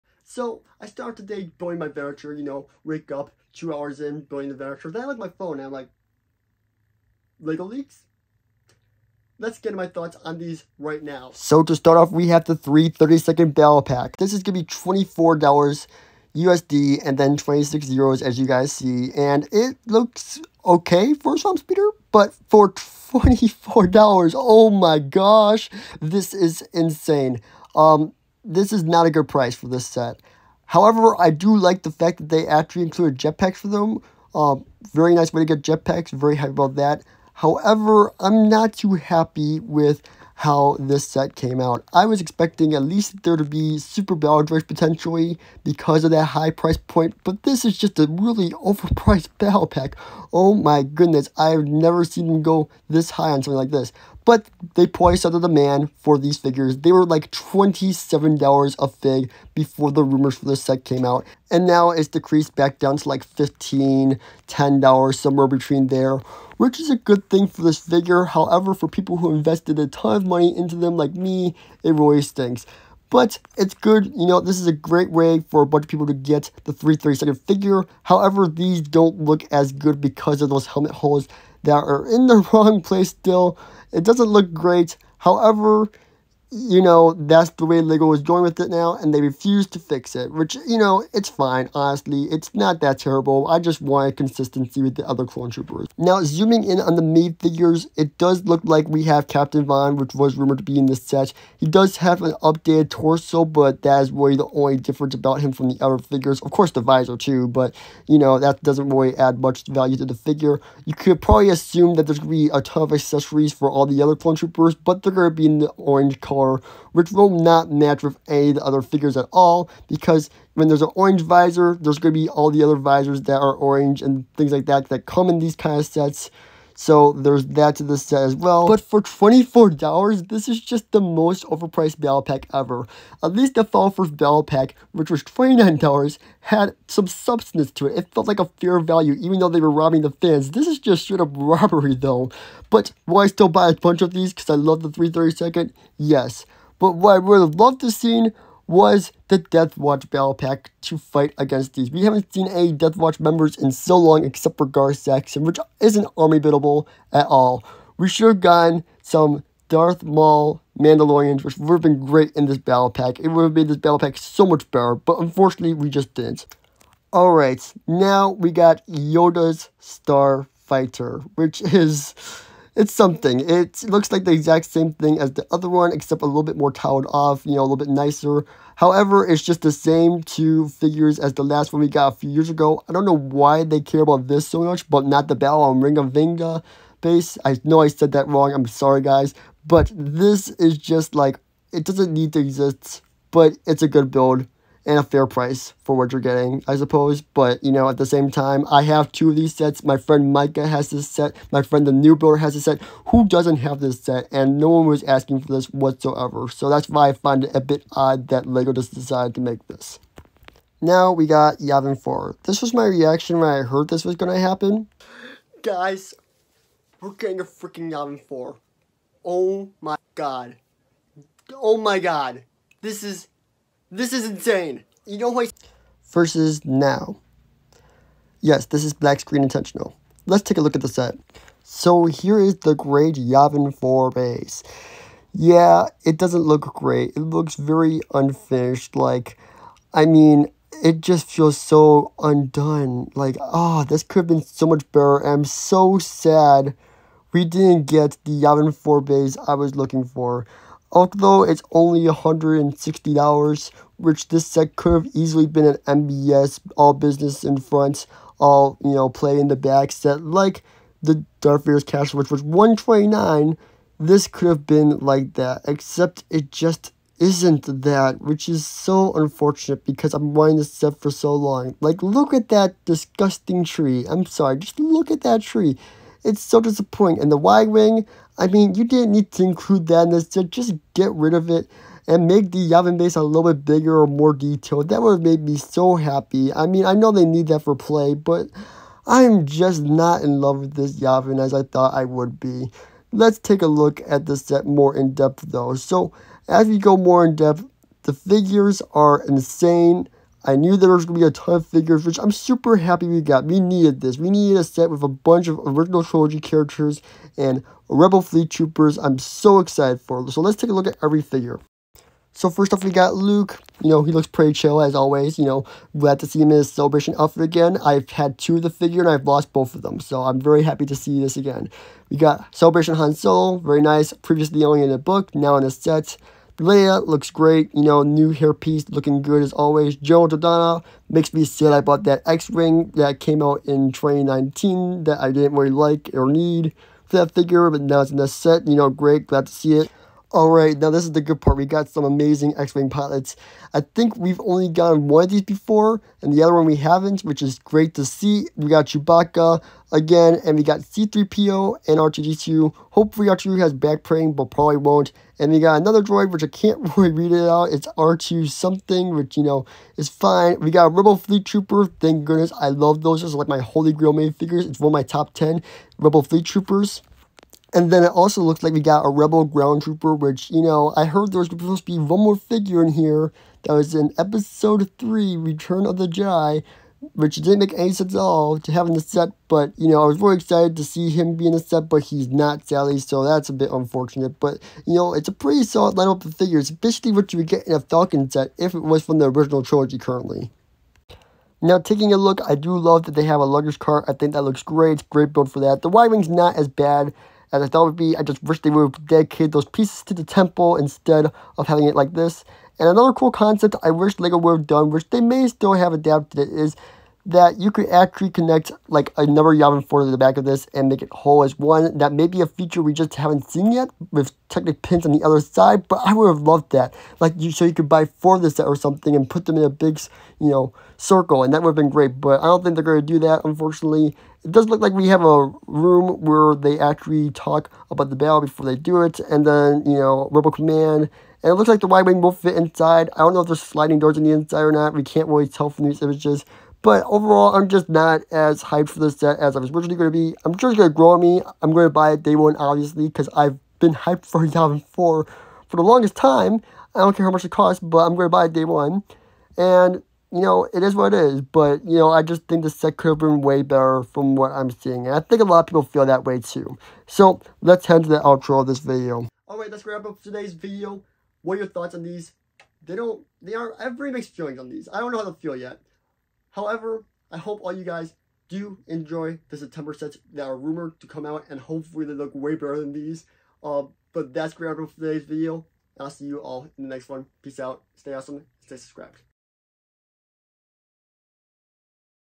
Lego YAVIN IV LEAK, 332nd BATTLLE PACK, B2 Battle Droids, - HUGE LEGO Star Wars UPDATE
Are you guys hyped for these New Lego STARWARS LEAKS!